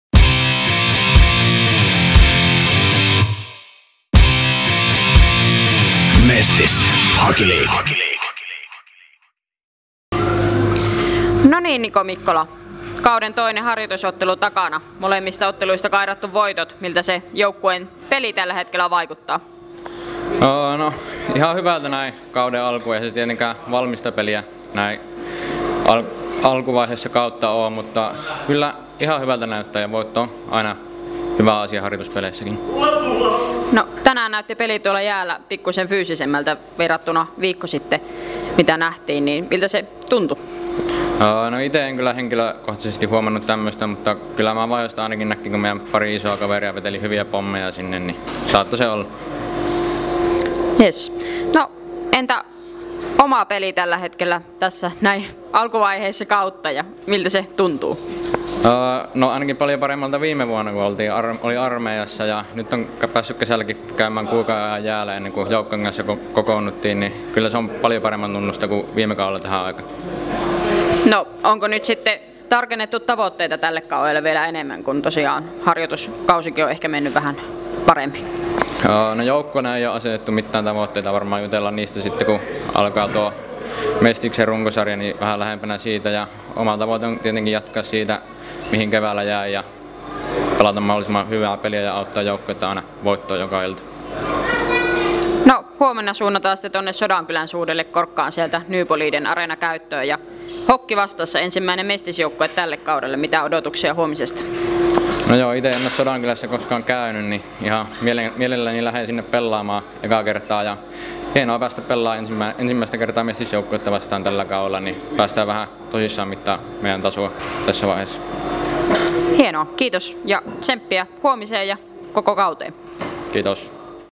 Postgame-haastattelu: Niko Mikkola (RoKi - Kalix HC 16.8.2019)
Niko Mikkolan mietteet 3-2 voittolaukausten jälkeen ruotsalaisjoukkue Kalixia vastaan päättyneen harjoitusottelun jälkeen. RoKin harjoitusottelut jatkuvat huomenna lauantaina, kun joukkue kohtaa Kajaanin Hokin Sodankylän uudella New Boliden Areenalla klo 17:00 alkaen.

Kuvaus ja editointi: Sami Kemppainen
Haastattelija: Sanna Lassila